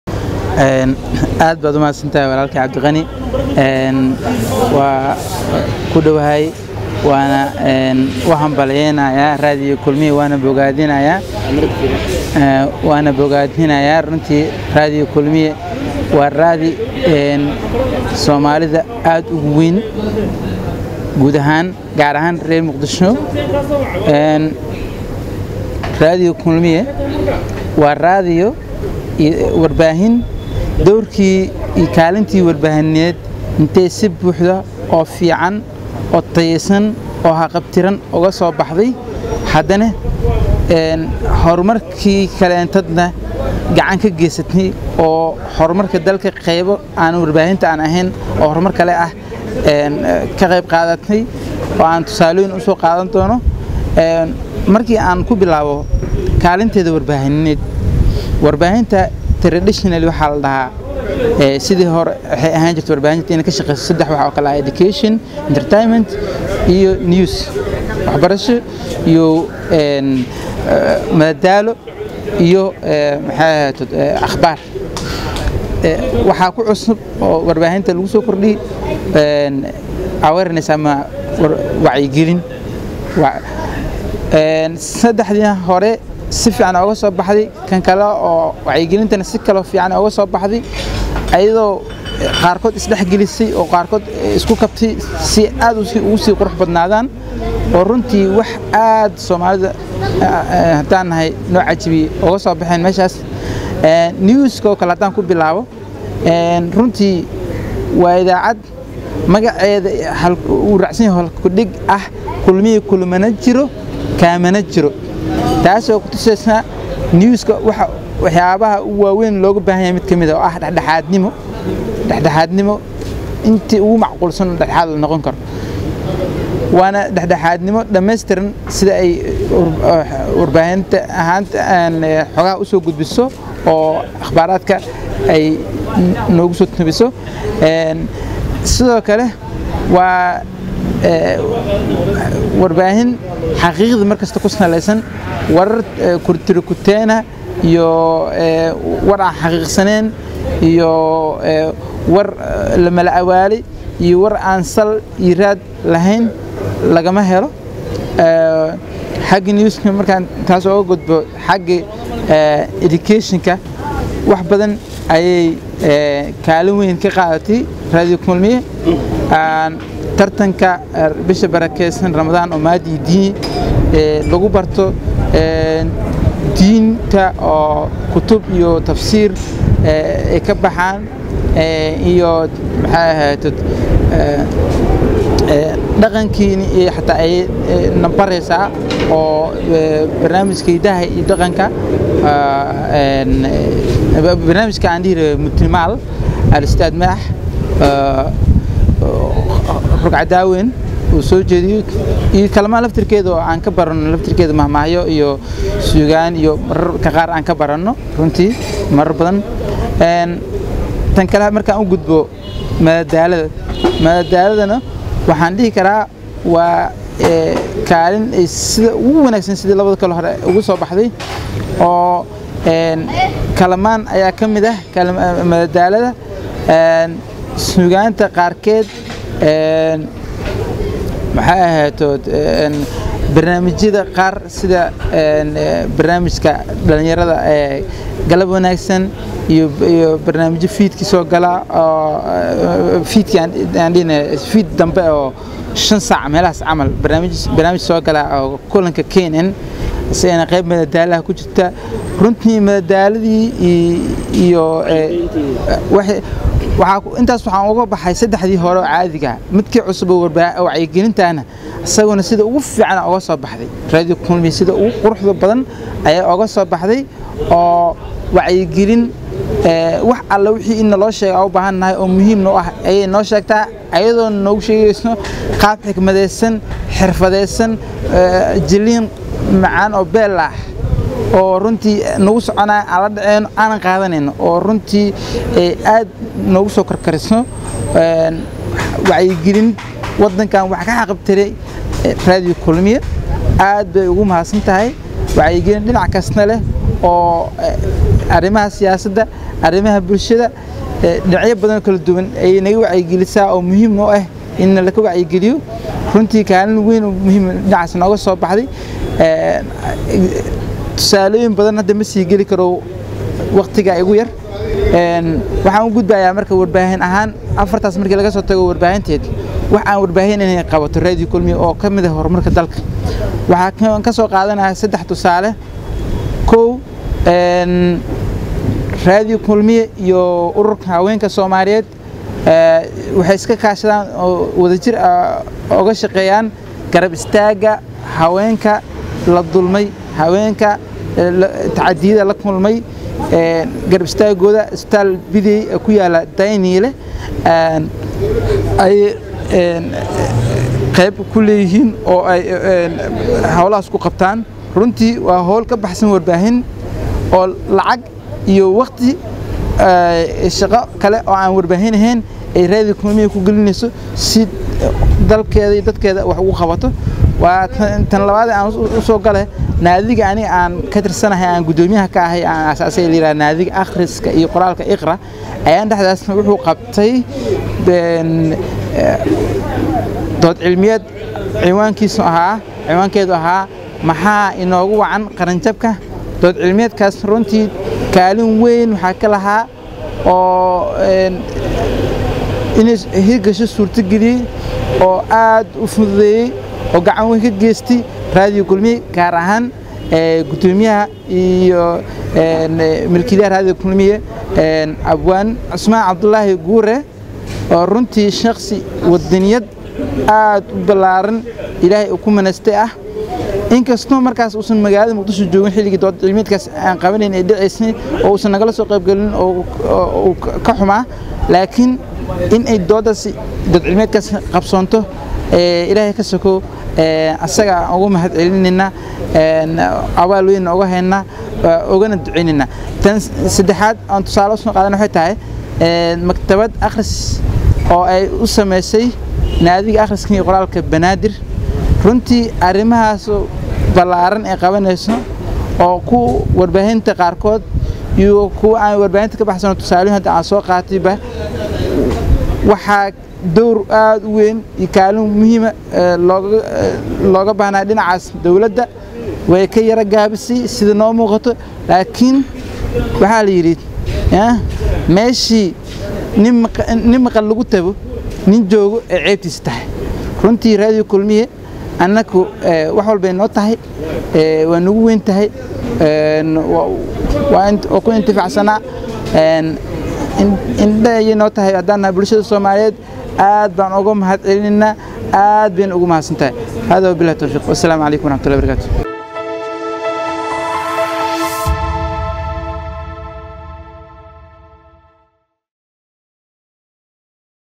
ABUUKAR CADDOW "KNN WAXAY QABATAY MA QARSOONA"
Idaacadda Kulmiye ee Muqdisho ayaa maanta sanad jirsatay waxaa sida ay bulshada ugu adeegtay ka hadlay Abuukar Cadow oo ah gudoomiyaha naadiga qoraalka iyo akhriska ee Iqra.